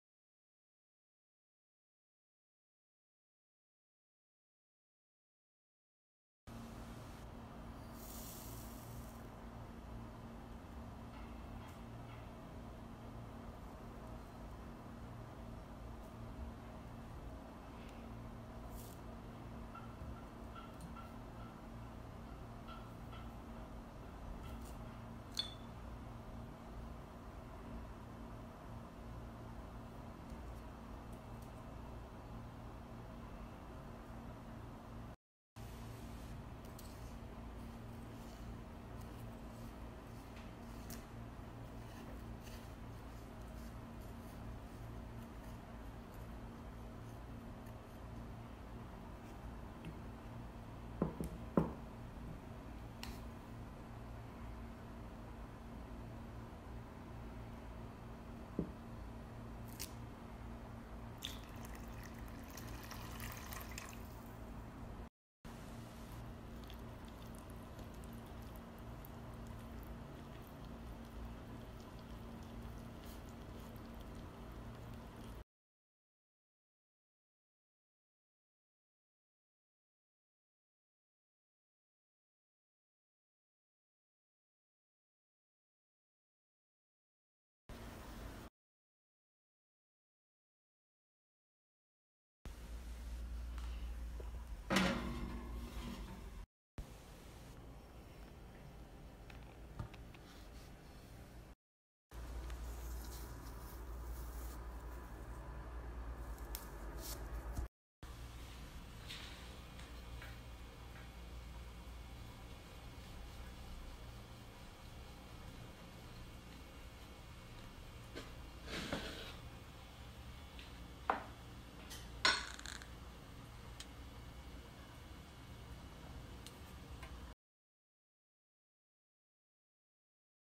توست بروتيني صحي للدايت
المقادير
 كوب دقيق بر نصف كوب شوفان مطحون ملعقة كل من سمسم شمار حبة البركة بذور الشيا بذور الكتان خميره ملعقة صغيره ملح حليب قليل الدسم دافئ ملعقتين بروتين بودر وممكن الاستغناء عنه ثلاثه ملاعق زيت زيتون